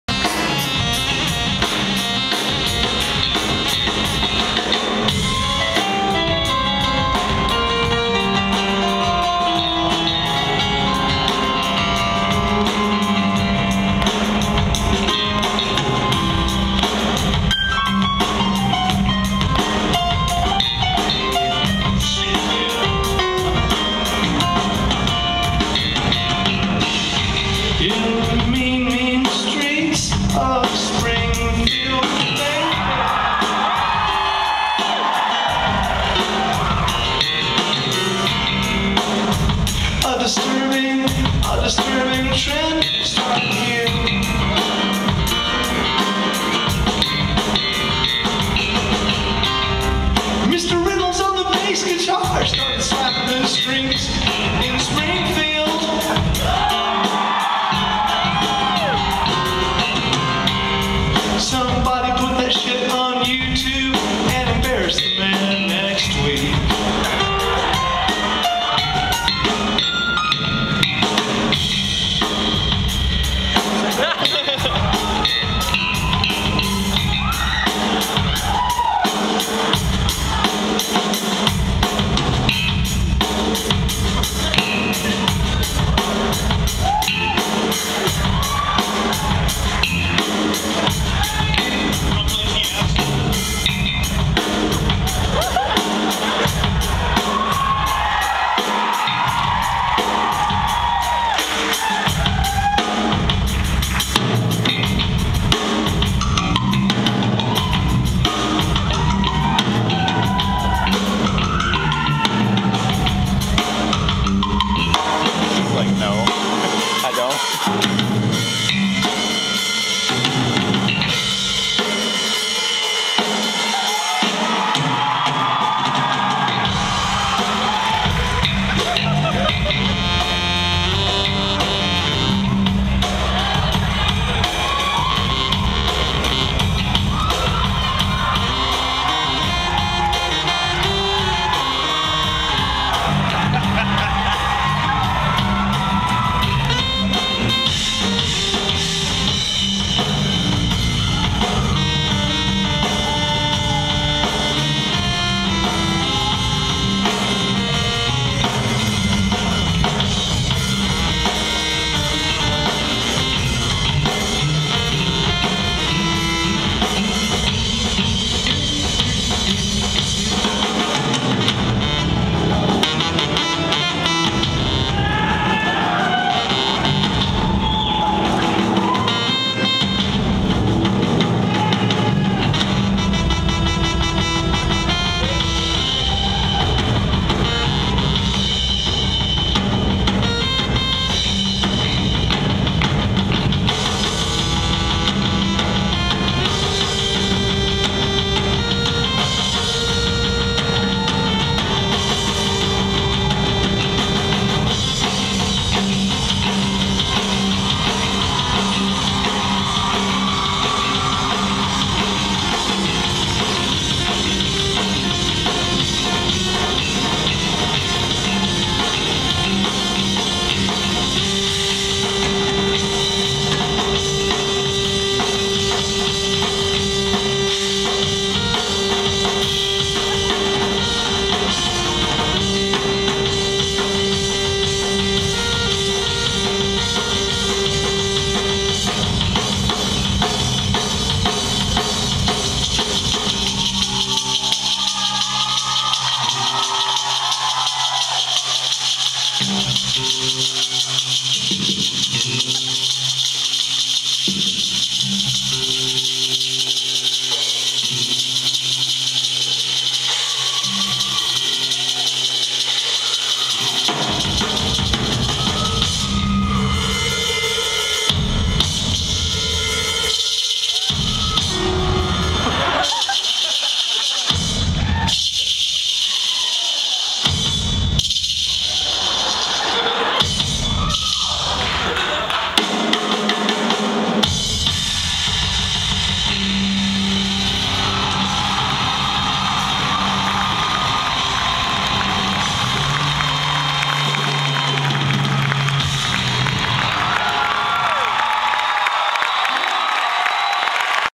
Ben Folds in Springfield, MO
Ben Folds sings a song for a girl's birthday.  He also trades places with the drummer toward the end of the song.  He says in the song to put the video on youtube to humiliate the bass player.